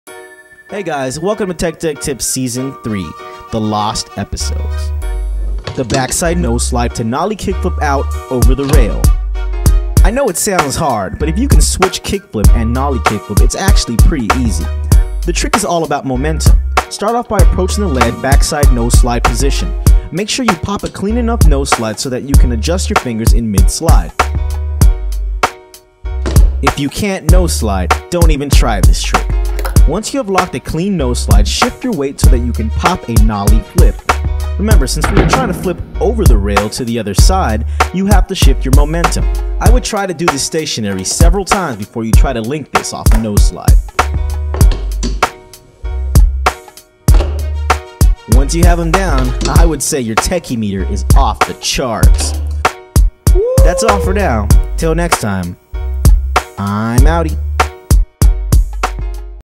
Tech Deck Tips_Season 3_Episode Eight
Learn How To Execute A b/s noseslide nollie- kickflip out, over the rail Right Here On Sk8rstv.com